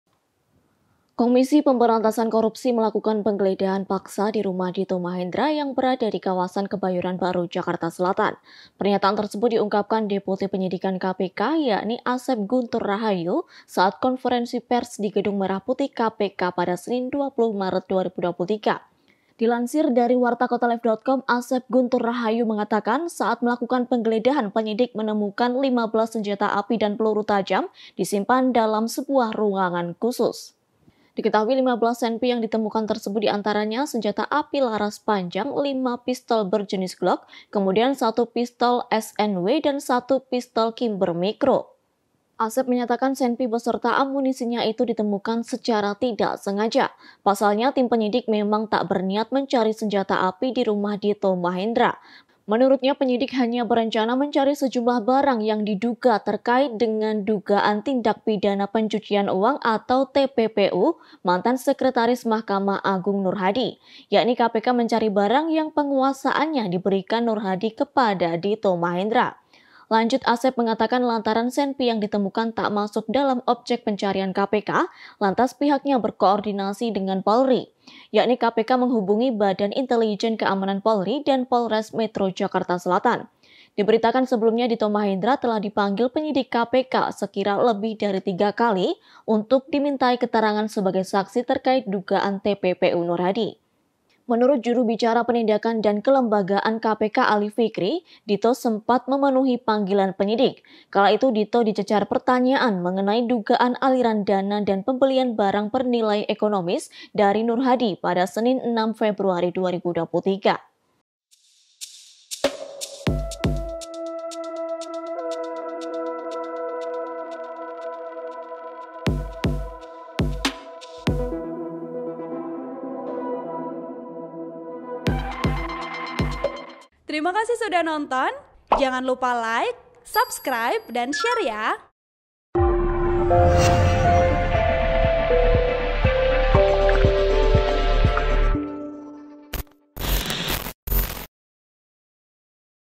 Geledah Rumah Dito Mahendra, KPK Temukan 15 Pucuk Senjata Api & Amunisinya, Ini Penjelasan Penyidik
TRIBUN-VIDEO.COM - Tim penyidik Komisi Pemberantasan Korupsi (KPK) menemukan 15 pucuk senjata api beserta amunisinya.

Senjata itu ditemukan di ketika KPK melakukan upaya paksa pemeriksaan rumah seorang pengusaha Mahendra Dito S atau Dito Mahendra.

Hal tersebut diungkapkan oleh Deputi Penyidikan KPK, Asep Guntur Rahayu dalam konferensi pers gedung Merah Putih KPK, Senin (20/3/2023).

Asep mengaku terlibat langsung dalam penggeladahan paksa di rumah Dito Mahendra di kawasan Kebayoran Baru Jakarta Selatan.

Senjata api dan amunisinya itu disimpan dalam sebuah ruangan khusus.

"Saya kebetulan juga ada di sana, itu betul dalam sebuah ruangan (khusus) ditemukan ada 15 pucuk itu lengkap dengan amunisinya, senjata api, peluru tajam," kata Asep.

Menurut Asep, tim penyidik KPK sebenarnya menemukan senjata itu dengan tidak sengaja.

Pasalnya mereka memang tidak berniat mencari senjata api di rumah Dito Mahendra.

Penyidik hanya bermaksud mencari sejumlah barang atau benda yang diduga terkait dengan dugaan tindak pidana pencucian uang (TPPU) mantan Sekretaris Mahkamah Agung (MA), Nurhadi.

KPK mencari objek benda yang penguasaannya diberikan oleh Nurhadi kepada Dito.

"Dalam proses penggeledahan kami harus melihat setiap sudut dari tempat yang digeledah.

Nah, saat itulah senjata-senyata itu ditemukan," ujarnya.

Baca juga: Nikita Mirzani Bocorkan Keberadaan Dito Mahendra Yang Masih Diburu KPK

Karena senjata tersebut tidak termasuk dalam objek pencarian KPK, pihaknya berkoordinasi dengan Polri.

KPK menghubungi Badan Intelijen Keamanan Polri dan Polres Metro Jakarta Selatan.

"Badan Intelijen Keamanan Polri kami hubungi terkait dengan masalah perizinan, karena senjata tersebut, kepemilikan senjata, izinnya dari Baintelkam," imbuh Asep seperti dilansir Kompas.com.

Adapun 15 senjata tersebut terdiri dari 8 senjata api laras panjang, 5 Pistol berjenis Glock, 1 Pistol S & W, dan 1 Pistol Kimber Micro.

Upaya paksa penggeledahan itu disaksikan sejumlah pihak di antaranya kerabat Dito, ketua RT setempat, asisten rumah tangga, hingga pihak keamanan kompleks.

Dito sebelumnya telah dipanggil penyidik KPK hingga lebih dari tiga kali untuk dimintai keterangan sebagai saksi mengenai dugaan TPPU Nurhadi.

Pada 6 Februari lalu, Dito memenuhi panggilan penyidik. Ia pun dicecar terkait dugaan aliran dana dan pembelian barang bernilai ekonomis oleh Nurhadi.

"Diduga (bersumber) dari pengurusan perkara di MA,” ujar Juru Bicara Penindakan dan Kelembagaan KPK Ali Fikri.

Usai menjalani pemeriksaan, Dito enggan menjawab pertanyaan wartawan, termasuk apakah dia menerima transfer sejumlah uang dari Nurhadi.

Saat itu, Dito dikawal sejumlah orang yang mengenakan pakaian serupa. Mereka mendampingi Dito dan sepanjang berjalan keluar dari gedung KPK.

Sebagai catatan Nurhadi merupakan terpidana kasus suap pengurusan perkara di Mahkamah Agung (MA).

Nurhadi dan menantunya, Rezky Herbiyono dinyatakan terbukti menerima suap dari dari Direktur Utama PT Multicon Indrajaya Terminal (MIT) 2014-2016 Hiendra Soenjoto terkait kepengurusan dua perkara Hiendra.

Selain itu, Nurhadi dan Rezky terbukti menerima gratifikasi Rp 13,787 miliar dari sejumlah pihak yang berperkara di tingkat pertama, banding, kasasi hingga peninjauan kembali (PK).

Nurhadi kemudian dijebloskan ke lembaga pemasyarakatan (Lapas) Sukamiskin, Bandung, Jawa Barat.

KPK lantas mengembangkan kasus ini dan mengusut dugaan TPPU. Sejumlah anggota keluarga Nurhadi diperiksa sebagai saksi.

Artikel ini telah tayang di WartaKotalive.com dengan judul Cari Bukti Pencucian Uang, KPK Temukan 15 Pucuk Senjata dan Amunisinya,

Penulis: Rusna Djanur Buana
Editor: Rusna Djanur Buana
Host: Adilla Risna
VP: Anggraini Puspasari